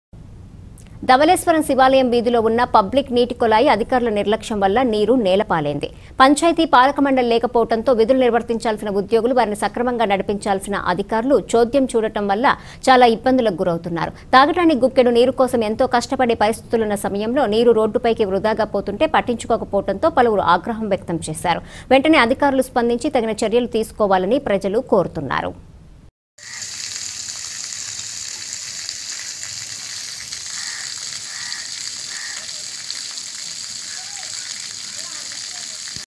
ధవళేశ్వరం శివాలయం వీధి లో ఉన్న పబ్లిక్ నీటి కొళాయి అధికారుల నిర్లక్ష్యం వల్ల నీరు నేల పాలైంది
ధవళేశ్వరం శివాలయం వీధి లో ఉన్న పబ్లిక్ నీటి కొళాయి అధికారుల నిర్లక్ష్యం వల్ల నీరు నేల పాలైంది.  పంచాయతీ పాలక మండలి లేకపోవడంతో విధులు నిర్వహించాల్సిన  ఉద్యోగులు వారిని సక్రమంగా నడిపించాల్సిన అధికారులు చోద్యం చూడటం వల్ల చాలా ఇబ్బందులకు గురవుతున్నారు. తాగడానికి గుక్కెడు నీరు కోసం ఎంతో కష్టపడి పరిస్థితులున్న  సమయంలో నీరు రోడ్డుపైకి వృధాగా పోతుంటే పట్టించుకోకపోవడంతో పలువురు ఆగ్రహం వ్యక్తం చేశారు  వెంటనే అధికారులు స్పందించి తగిన  చర్యలు తీసుకోవాలని ప్రజలు కోరుతున్నారు.